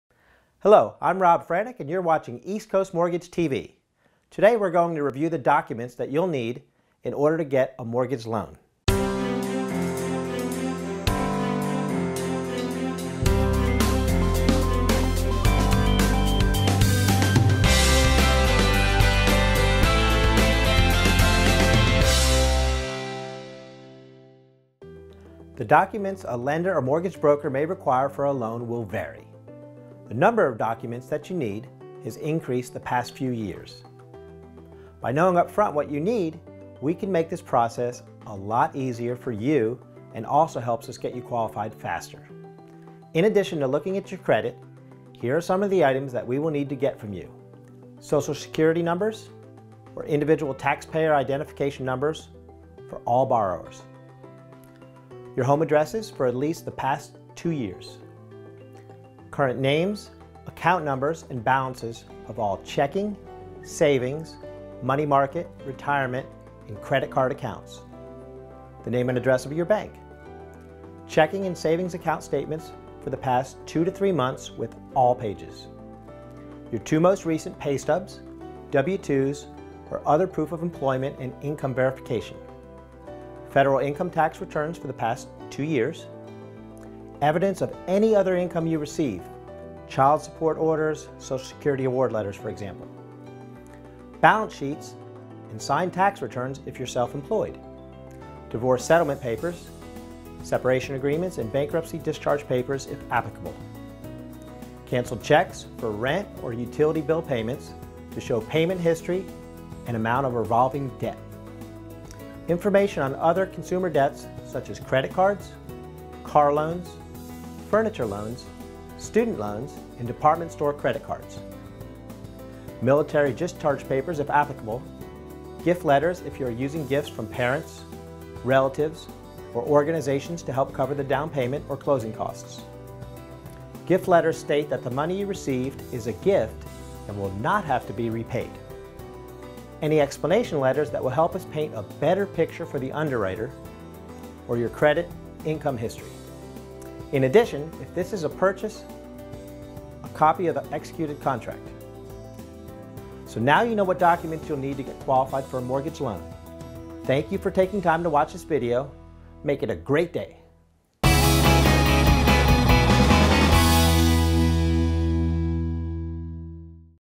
Charlotte Mortgage Lender Reviews Documents Required For Mortgage Loan
Charlotte Mortgage Lender East Coast Mortgage Lending President Rob Franek goes over the documents that are required to get a mortgage loan.The documents a lender or mortgage broker may require for a loan will vary.   The number of documents that you need has increased the past few years.  By knowing up front what you need, we can make this process a lot easier for you and also helps us get you qualified faster.

All music licensed by SmartSound